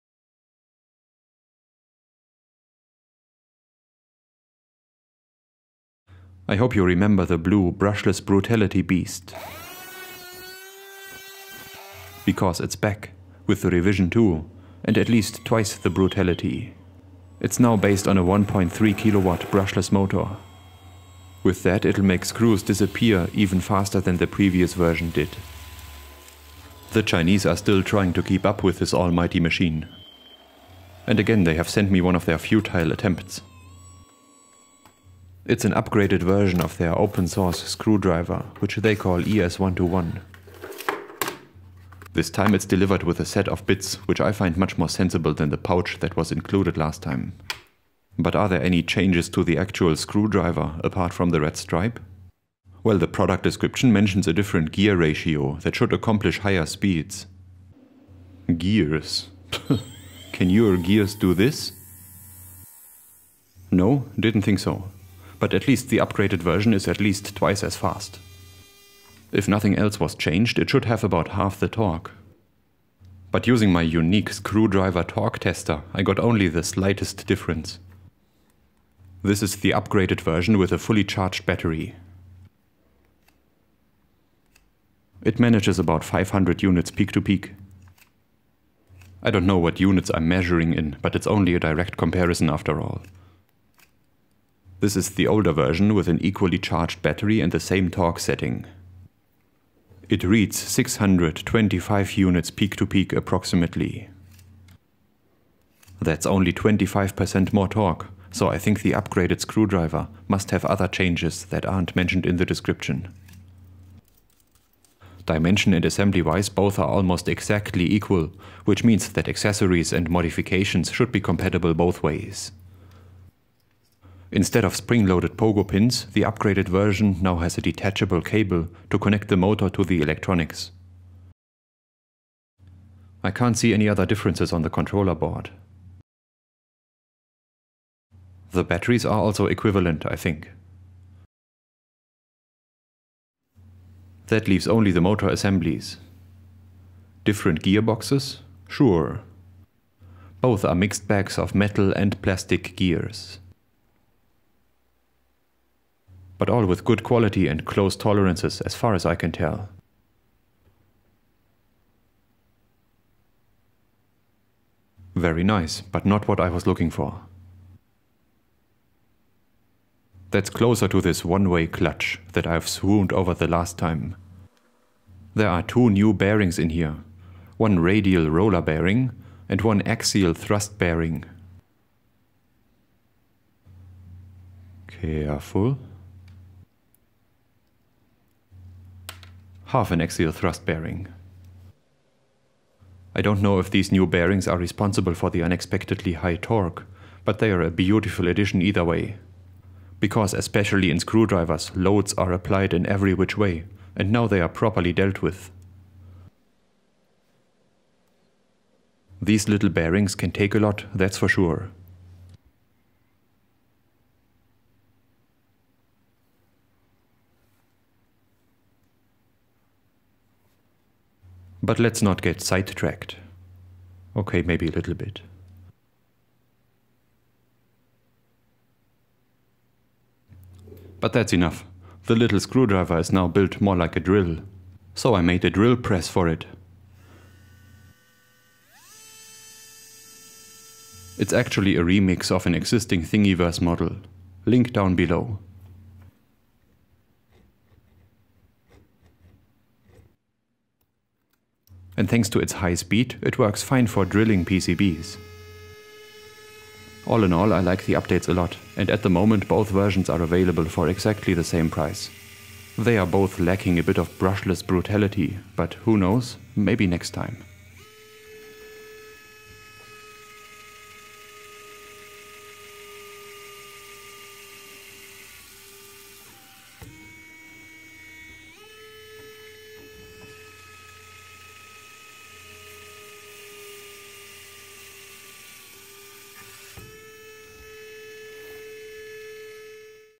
ES121 - Better, faster revision of the ""Open Source Screwdriver""!
My last video about the ES120 was very well received, so the manufacturer sent me the new, upgraded version, too. e-Design is known for the coolest open source gadgets like the TS100 soldering iron, different oscilloscopes and now the wildly popular motor screwdrivers!

This upgraded version is called ES121 and it has a few more tricks up its sleeve than just a different gear ratio.